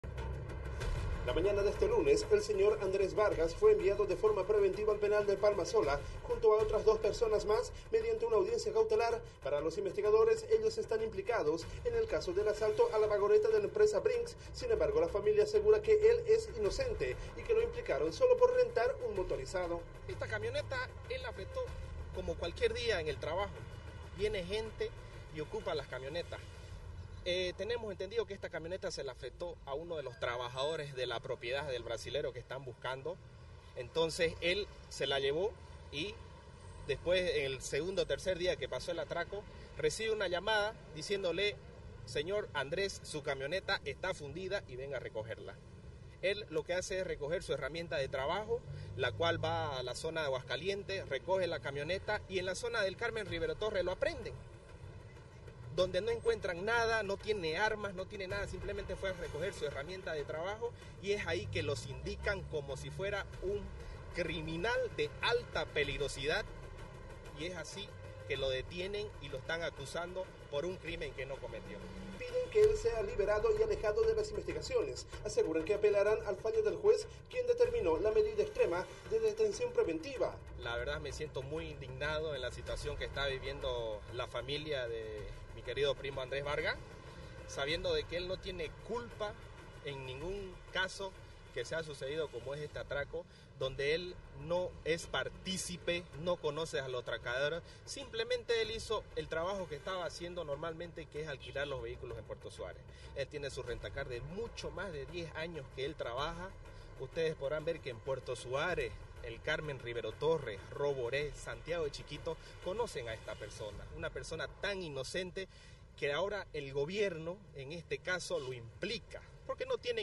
Familiares afirman inocencia de detenido
Familiares de uno de los detenidos por el atraco al camión de remesas de Brinks, aseguran que su allegado es inocente, que su error fue haber alquilado un motorizado que luego los antisociales lo utilizaron para el robo.

"Su error fue alquilarles un auto”